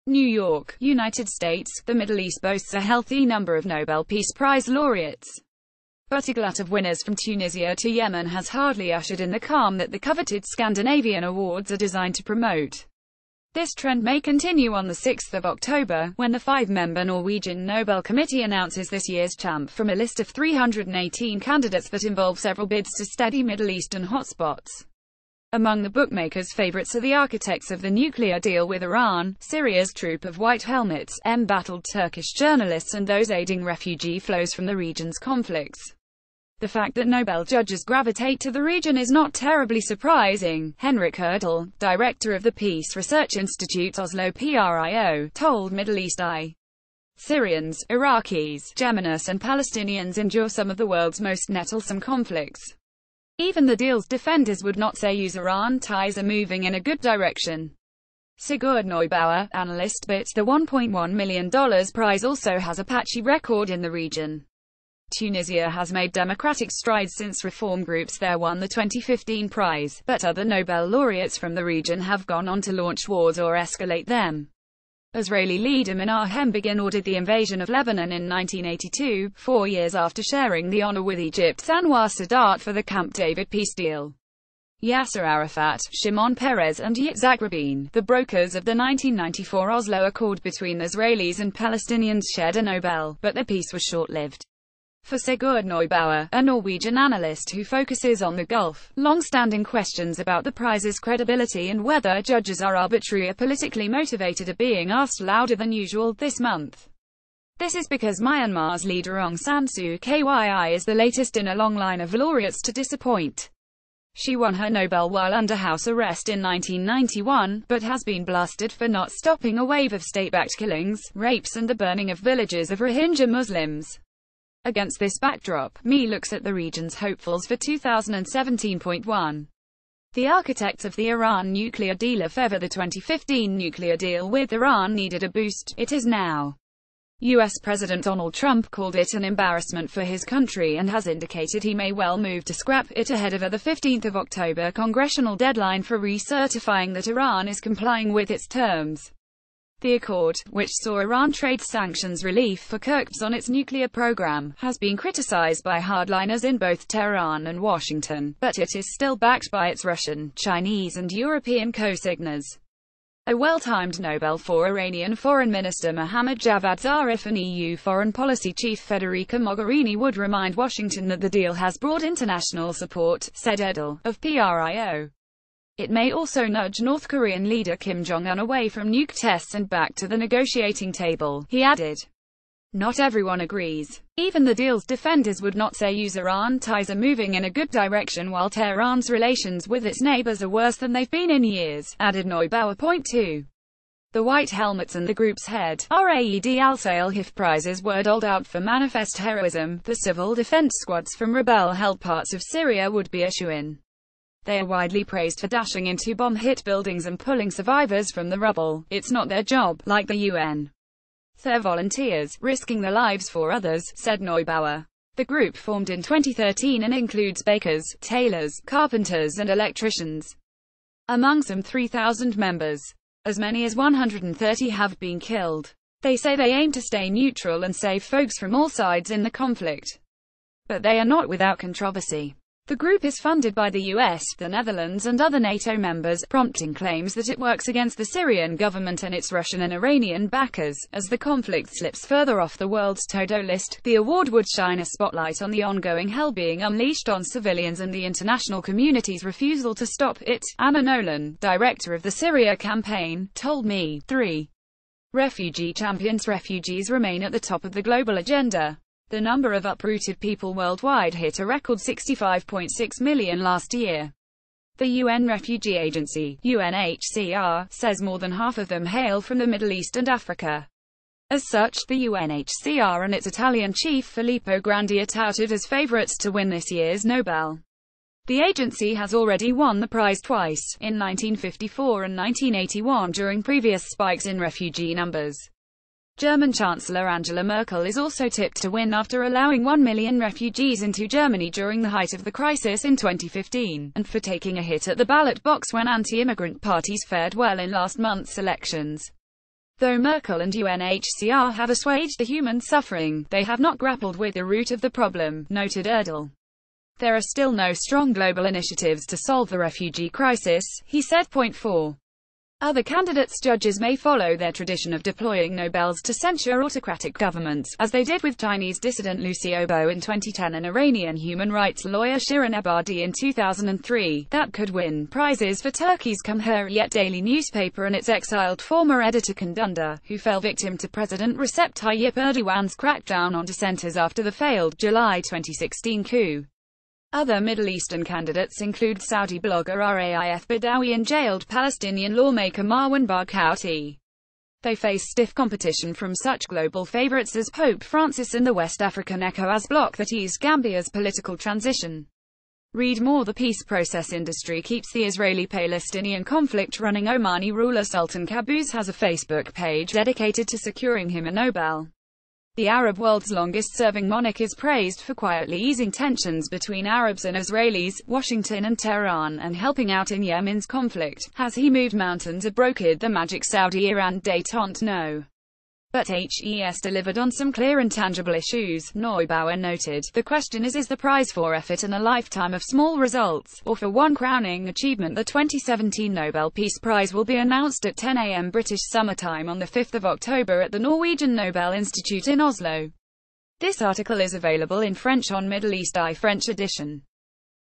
Mideast peace bids top list of likely nobel winners
NEW YORK, United States – The Middle East boasts a healthy number of Nobel Peace Prize laureates. But a glut of winners from Tunisia to Yemen has hardly ushered in the calm that the coveted Scandinavian awards are designed to promote. This trend may continue on 6 October, when the fivemember Norwegian Nobel Committee announces this year’s champ from a list of 318 candidates that involve several bids to steady Middle Eastern hotspots. Among the bookmakers’ favourites are the architects of the nuclear deal with Iran, Syria’s troupe of White Helmets, embattled Turkish journalists and those aiding refugee flows from the region’s conflicts. The fact that Nobel judges gravitate to the region is “not terribly surprising,” Henrik Urdal, director of the Peace Research Institute Oslo PRIO, told Middle East Eye. Syrians, Iraqis, Yemenis and Palestinians endure some of the world’s most nettlesome conflicts. Even the deal’s defenders would not say USIran ties are moving in a good direction. Sigurd Neubauer, analystBut the $1.1m prize also has a patchy record in the region. Tunisia has made democratic strides since reform groups there won the 2015 prize, but other Nobel laureates from the region have gone on to launch wars or escalate them. Israeli leader Menachem Begin ordered the invasion of Lebanon in 1982, four years after sharing the honour with Egypt’s Anwar Sadat for the Camp David peace deal. Yasser Arafat, Shimon Peres and Yitzhak Rabin, the brokers of the 1994 Oslo accord between...